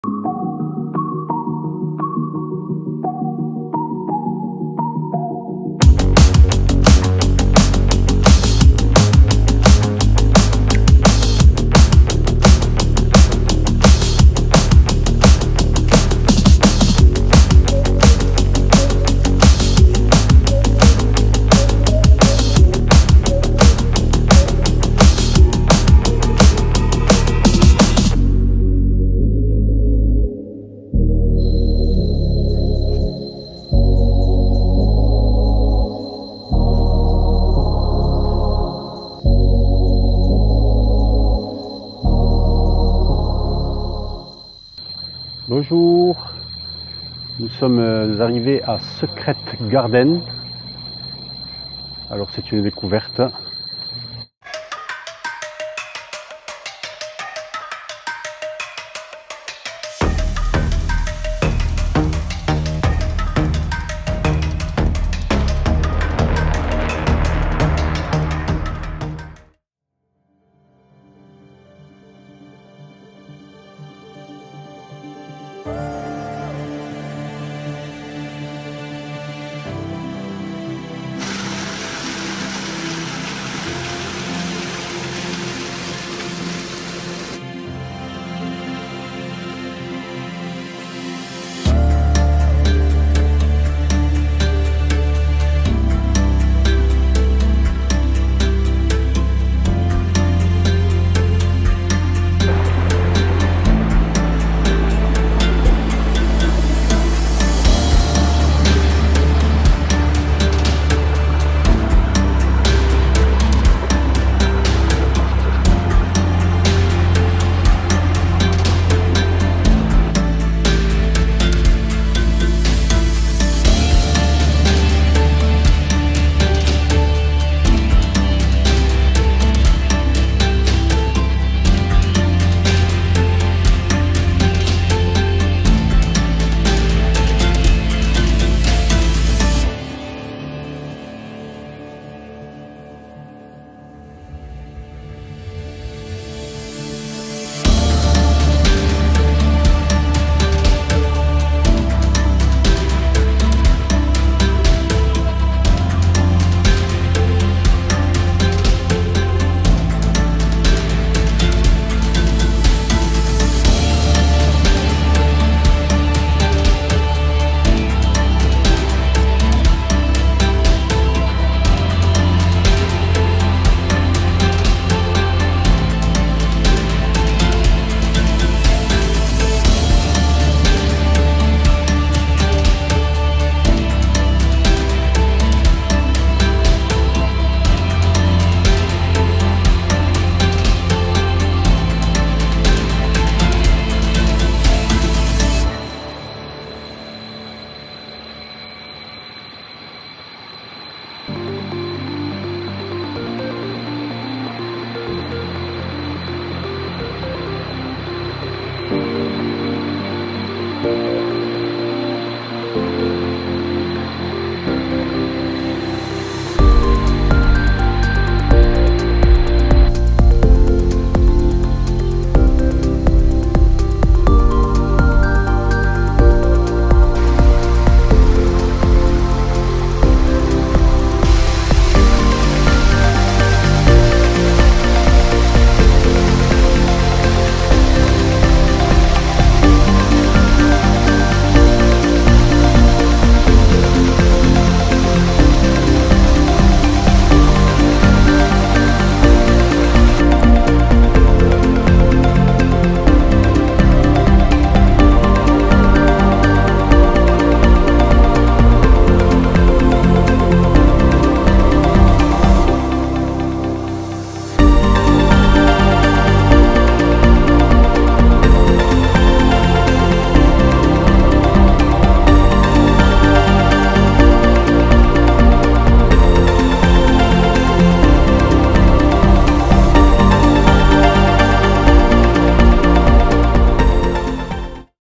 Secret garden Koh samui Thailande
Venez découvrir secret garden sur l’île de Koh samui en Thaïlande